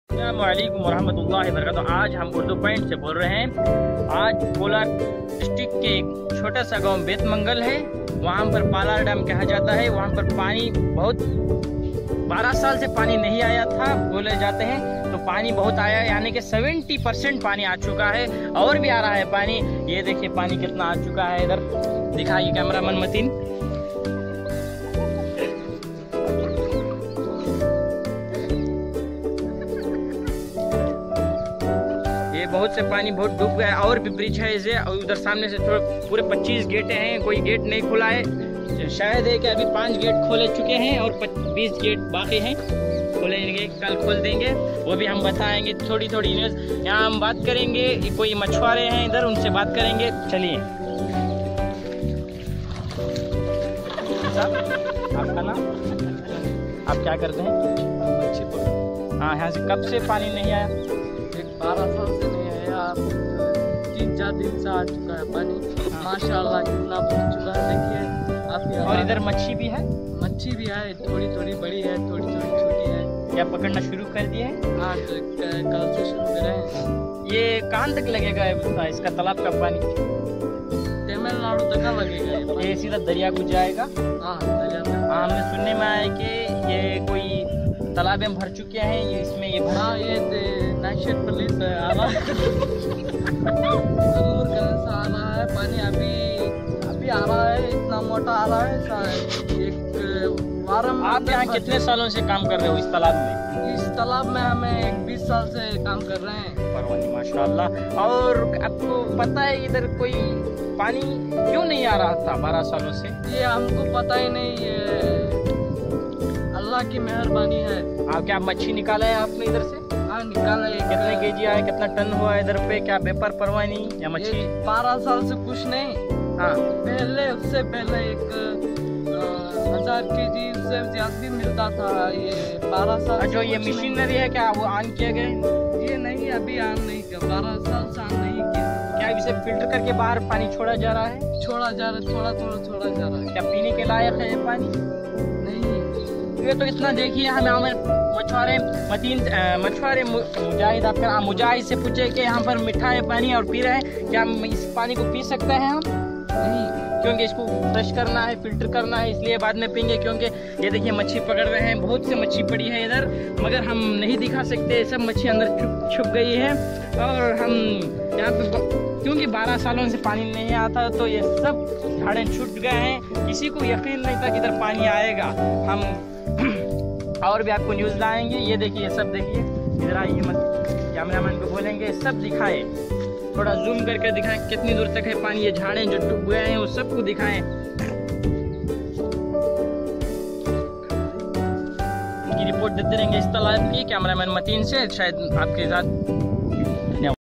Bethamangala palar dam special report
special report about bethamangala palar dam located in karnataka kolar [dt] india